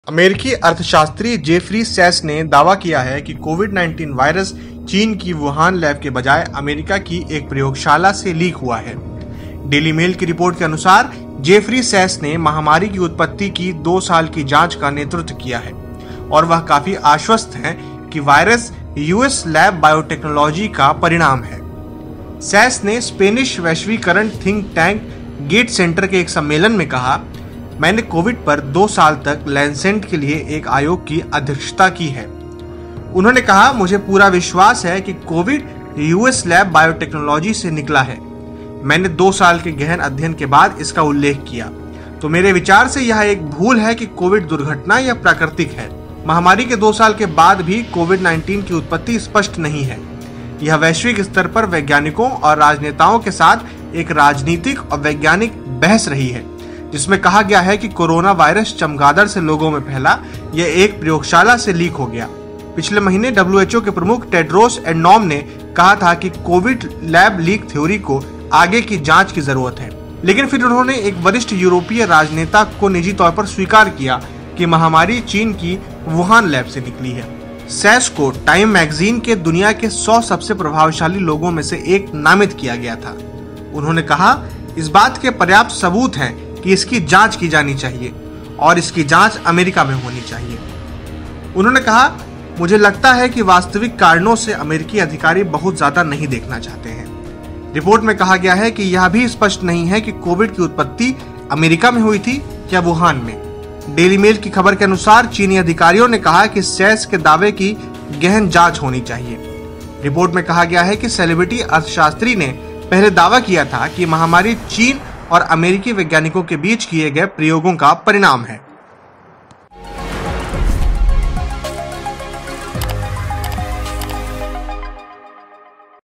Covid 19 Leak News: US Professor का दावा Wuhan नहीं, American Lab से लीक हुआ था Coronavirus
अमेरिकी अर्थशास्त्री जेफरी सैस ने दावा किया है कि कोविड-19 वायरस चीन की वुहान लैब के बजाय अमेरिका की एक प्रयोगशाला से लीक हुआ है। डेली मेल की रिपोर्ट के अनुसार, जेफरी सैस ने महामारी की उत्पत्ति की दो साल की जांच का नेतृत्व किया, वह 'काफी आश्वस्त' थे कि वायरस 'यूएस लैब बायोटेक्नोलॉजी' का परिणाम था। सैस ने स्पेनिश वैश्वीकरण थिंक-टैंक गेट सेंटर के एक सम्मेलन में कहा, 'मैंने कोविड पर दो साल तक लैंसेट के लिए एक आयोग की अध्यक्षता की है।'